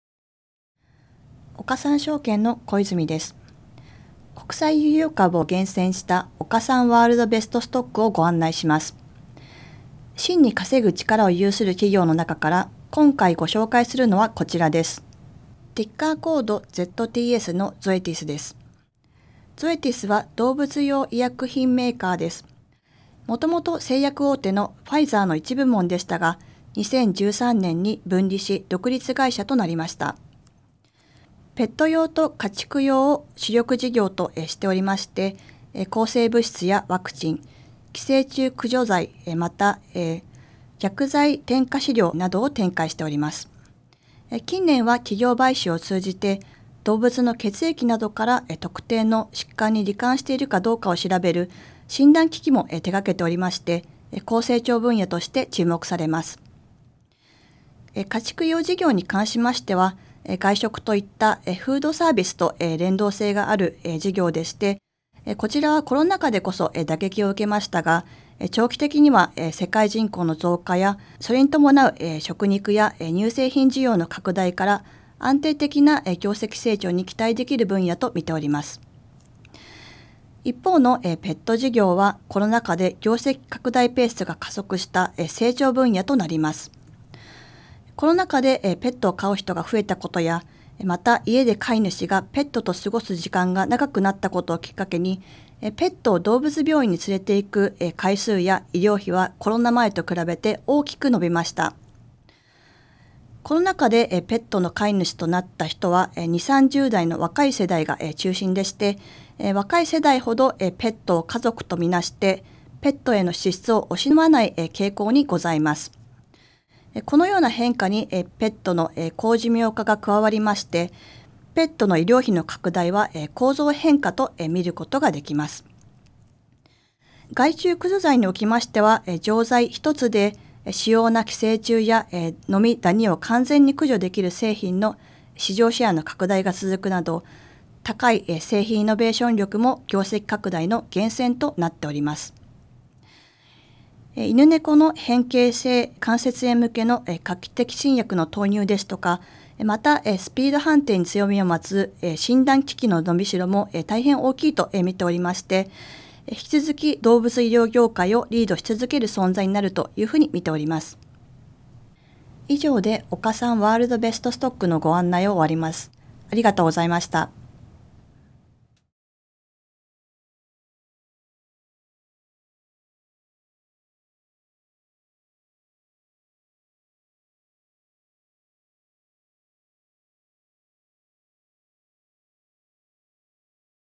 岡三 World Best Stock ～ゾエティス（ZTS） ～【岡三証券】WEBセミナー
弊社が厳選した国際優良株をご紹介する「岡三WorldBestStock」
略して「岡三WBS」

今回は「 ゾエティス（ZTS）」を
岡三証券・小泉めぐみシニアストラテジストが解説します。

なお、動画内でご紹介しきれなかった情報は
岡三証券ホームページ内に掲載している
「岡三WorldBestStock」のPDFをご覧ください。

※動画の最後に重要なお知らせがございます。
　どうぞ最後までご確認ください。

資産運用のご相談なら岡三証券

【その他Web動画セミナーも、岡三証券ホームページ内にて随時更新中】